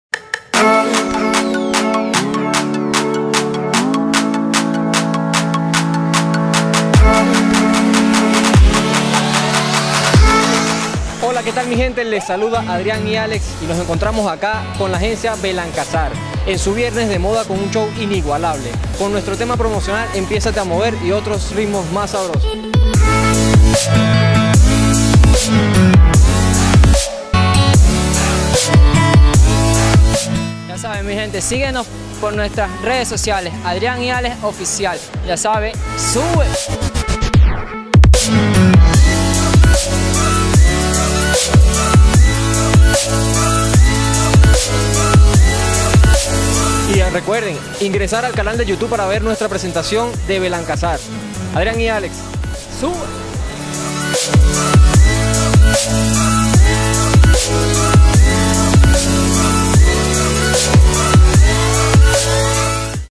Adrian y Alex, artistas invitados en Viernes de Moda Belankazar con Tiendas Melao- 2/09/2016
Desfile de Modas con los Modelos Belankazar y los diseños tiendas Melao, en el marco de los Viernes de Moda con el Centro Sambil Caracas. Desfile realizado en 2 de septiembre de 2016.